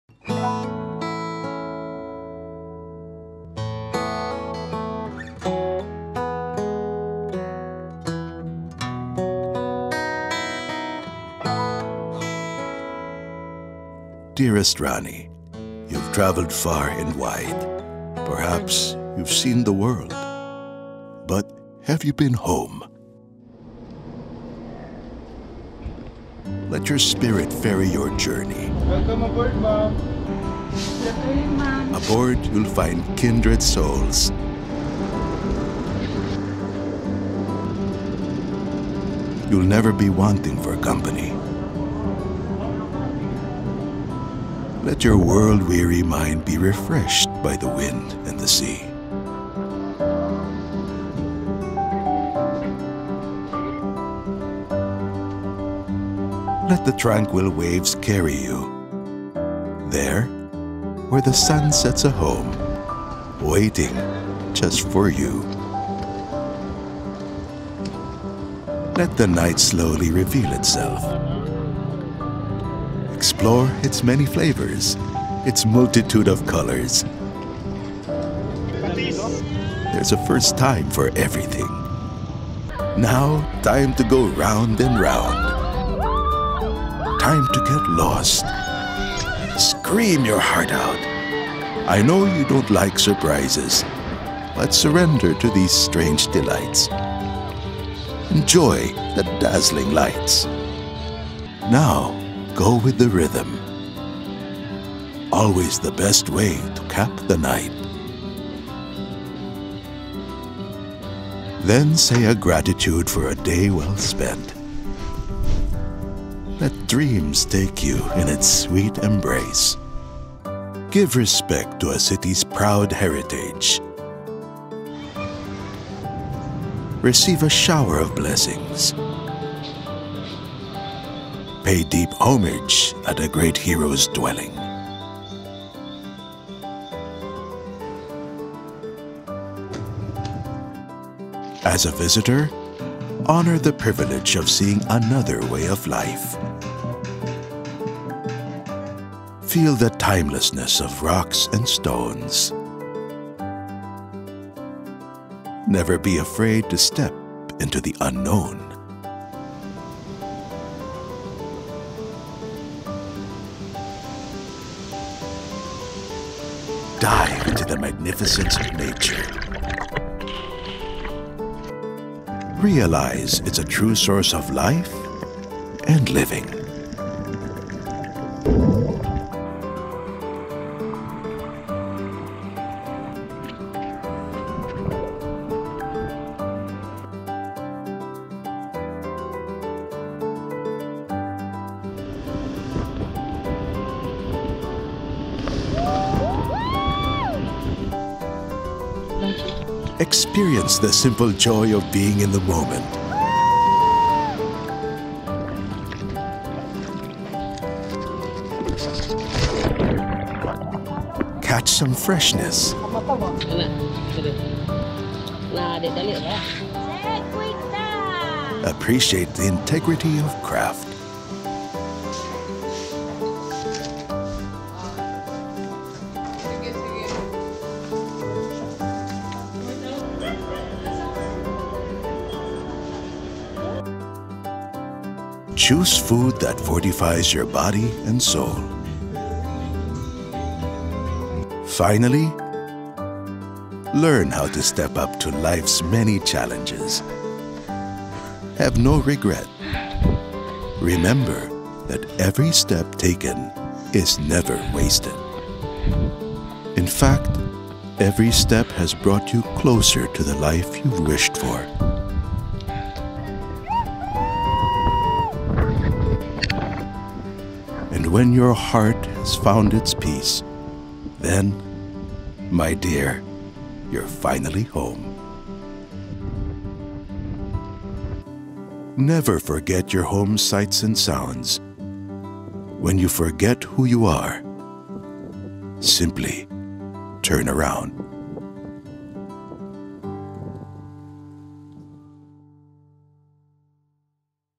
Dapitan Souvenir for the Soul
Food for the body, mind, and soul. What better place to find all three than in Dapitan?

Take a nostalgic retreat in this gem of a destination known for its art, culture, and historical scene. Discover Dapitan with FastCat.